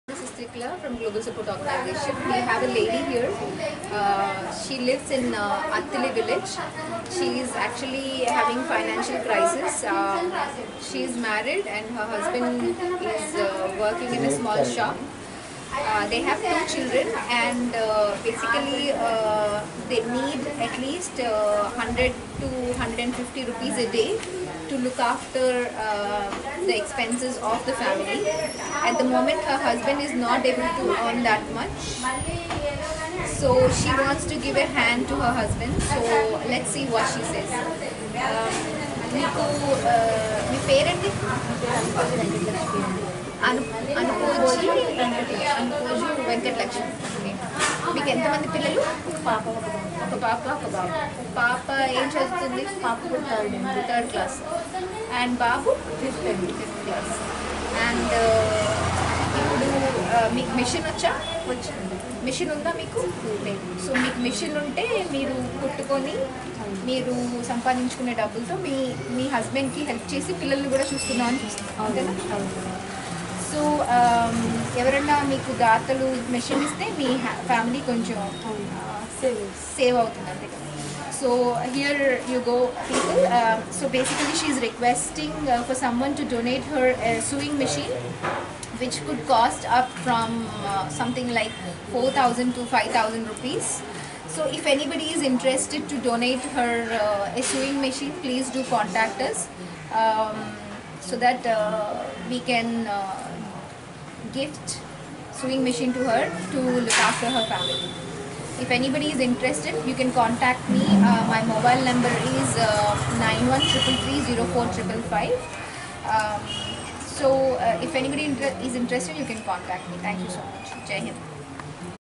Sponsor a Sewing Machine to a Lady from Poor Family
Hi Everyone,
Today i met Mrs Venkata Lakshmi from Attili, she belongs to a poor family. Currently her husband has bad health conditon related to heart and recently also met with an accident. Her husband is working in a small shop in which the income is uncertain and it totally depends on sale each day with that low income Venkata Lakshmi & her husband are unable to pay her husbands medical bills along with maintenance of their 2 young children. venkata Lakshmi knows stitching so, if anyone can sponsor her a sewing machine which can cost between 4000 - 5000 Indian Rupees, she will be able to earn and support her husband to pay towards their livelihood. Please think and come forward as your help can feed a family of Four. Kindly share as it reaches someone who can help. Jai Hind!
Kind Regards,
Trikala Chenna,
Founder - Global Support Organisation